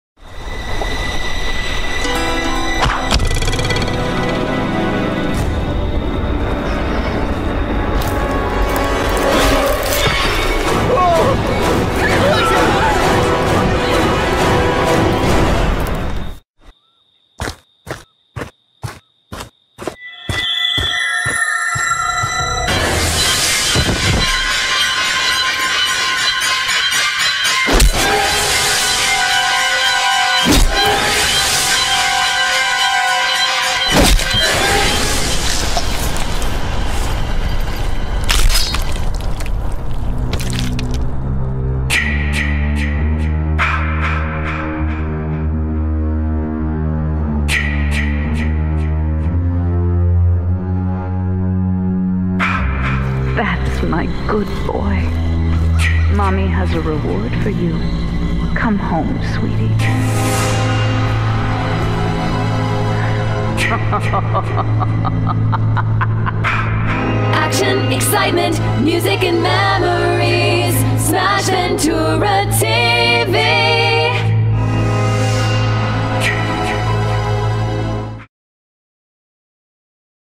Smash Ventura TV (Returning August 28th at 8 pm est!!!!) Friday The 13th The Game Parody!
Smash Ventura TV returns August 28th at 8 pm est!
All kinds of great nostalgic fun and features!
Retro mercials, What's on TV?, Movie Music, Video Game Vault, Special Interview Guest and much more!
Join me here every Saturday night at 8 pm est beginning Saturday August 28th and we all can chat and hang out during the show :)
Thanks everyone for the support it means a lot :)
"See ya in the past..."
- SV